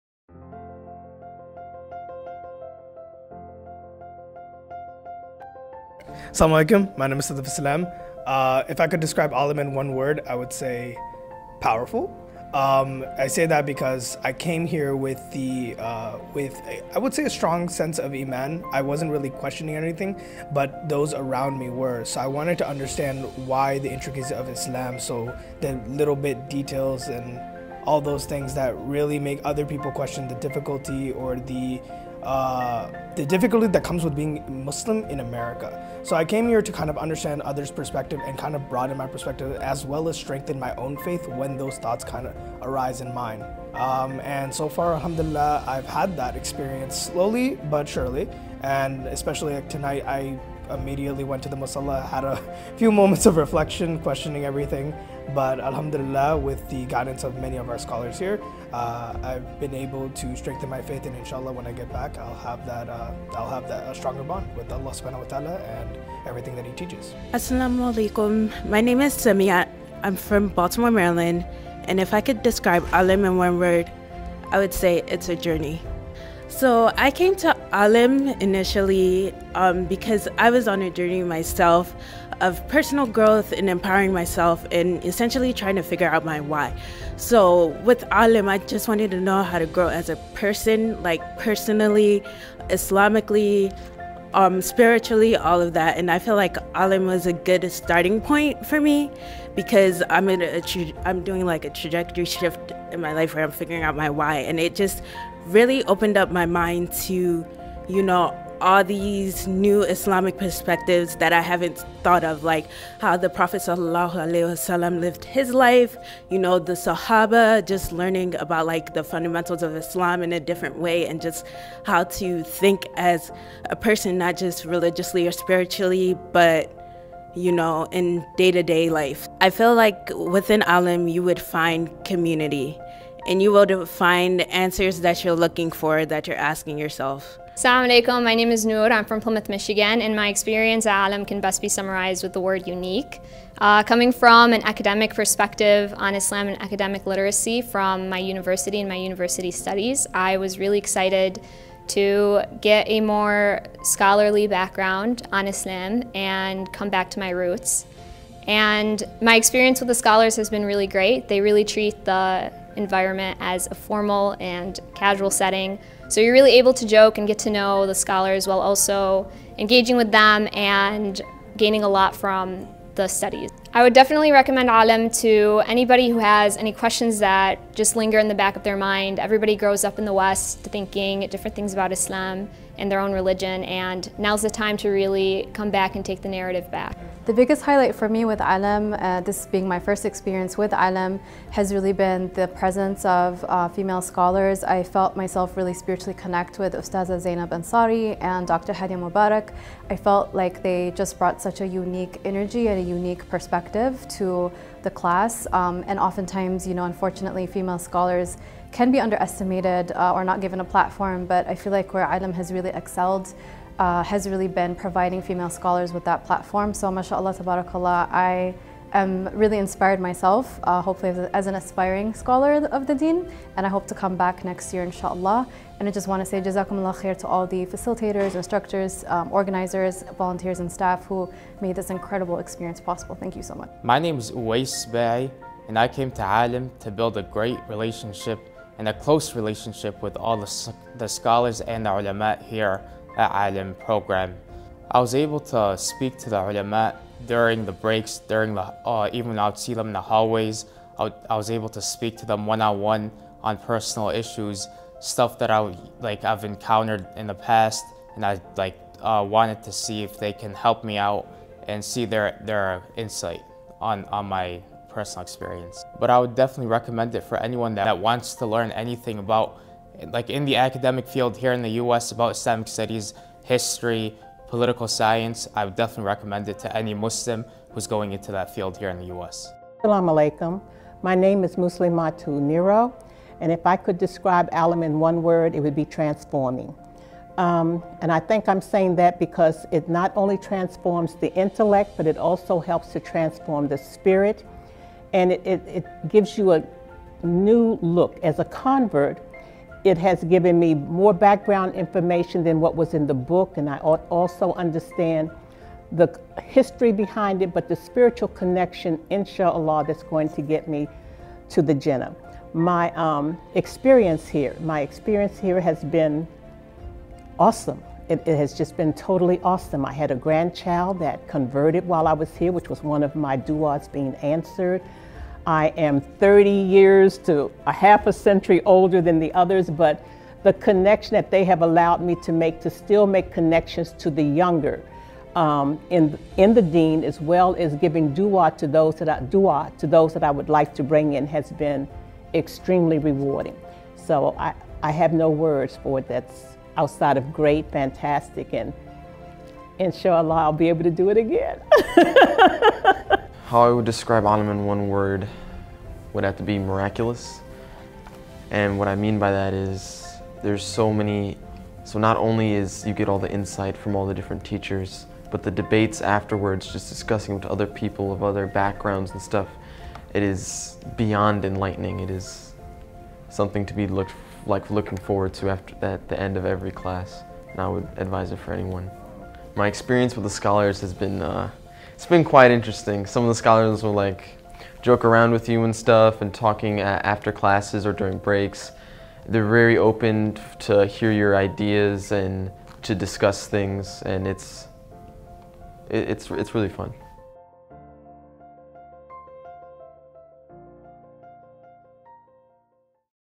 ALIM Summer Program: 2021 Student Testimonials
Hear from 2021 ALIMni about their experiences at the ALIM Summer Program.